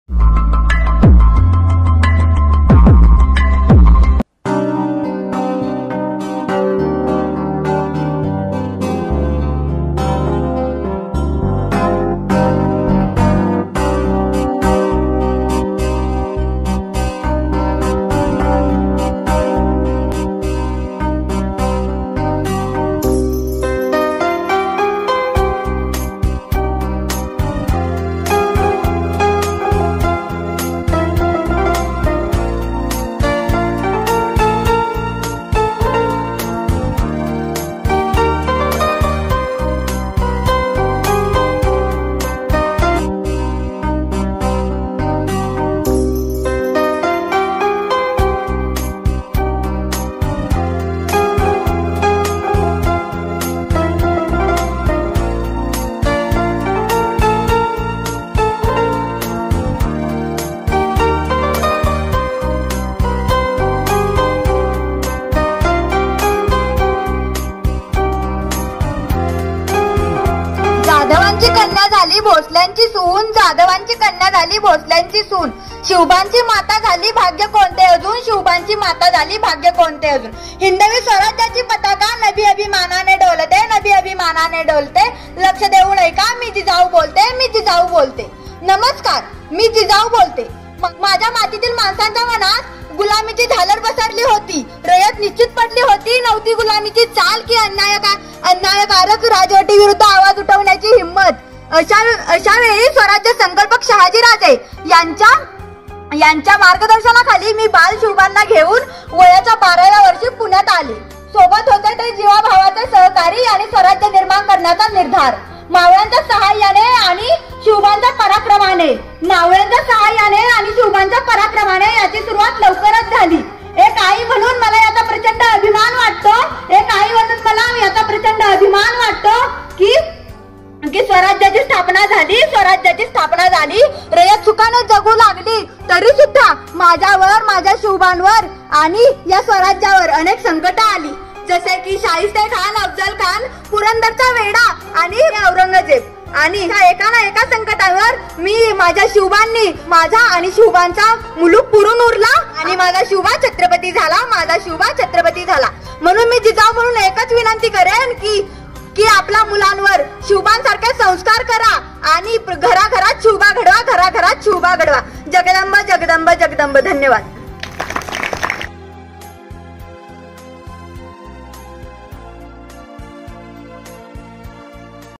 मी जिजाऊ बोलतेय... विद्यार्थी मनोगत कु. श्रेया मोहिते
श्री अंबिका विद्यामंदिर, तोंडोली. ता. कडेगांव. जि. सांगली.(श्री.कुंभार डी.एस. )